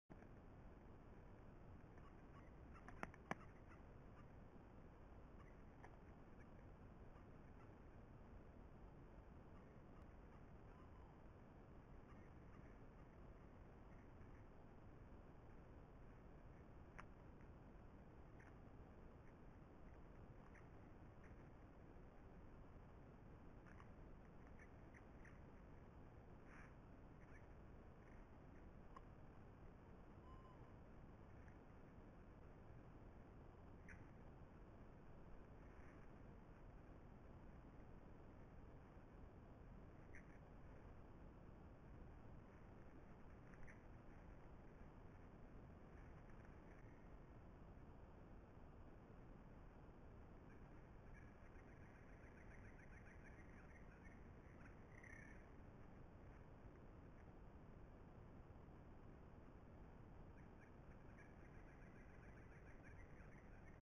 Zorrillo explorador
En estos días empiezan a independizarse y son fáciles de ver a pleno día (el hambre aprieta).